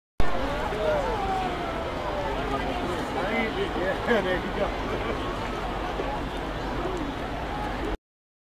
Chicago Cubs vs. Los Angeles Dodgers 7/11/10 part 13
Just watching the Chicago Cubs vs Los Angeles Dodgers at Dodger Stadium